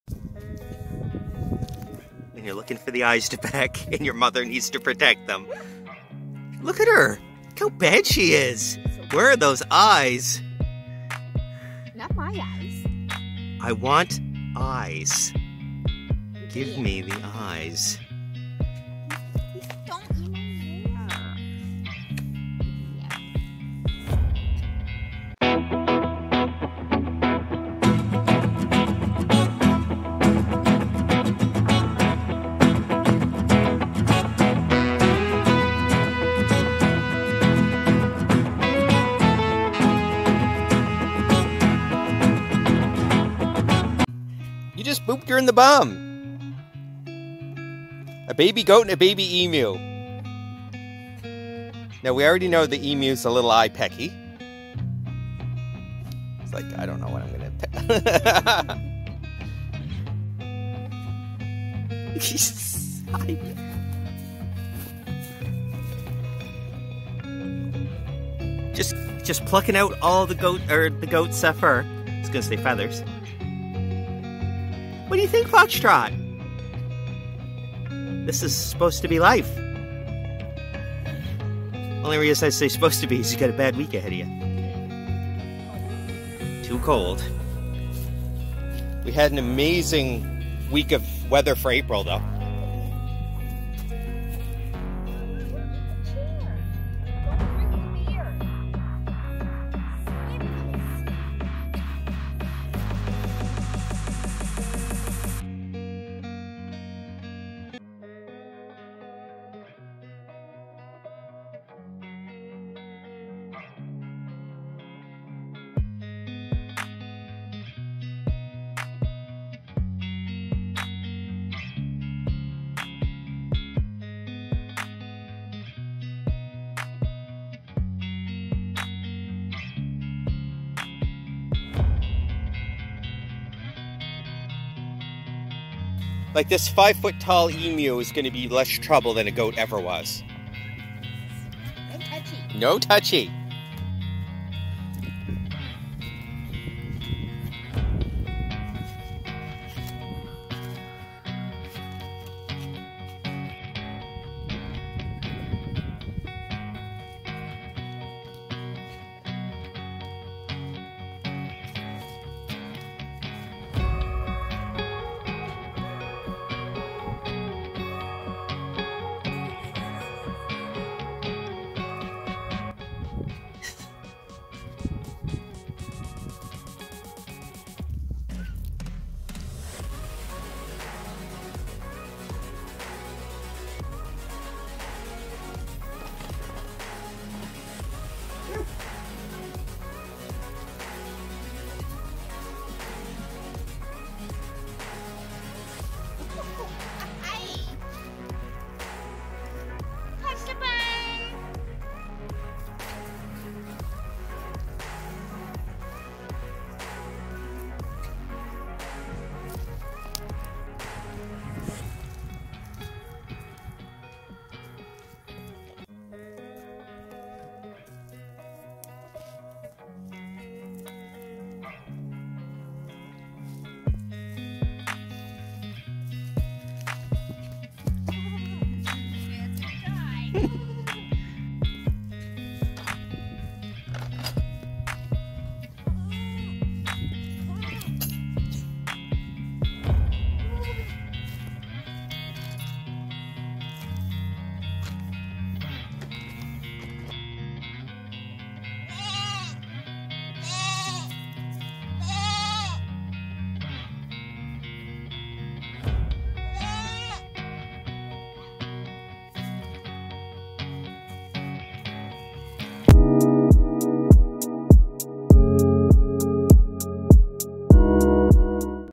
Baby Emu loves her baby goats
Our baby emu, Foxtrot, got to experience her very first day outside. And in the same pen with her was a few baby goats. Foxtrot was raised in a house with bottle baby goats, but there was always a fence dividing them. Well today she got to play with them with no boundaries and she really seemed to enjoy the experience. And our baby emu loves her baby goats!

So come check out what happens with a baby emu meets her baby goats in person for the very first time!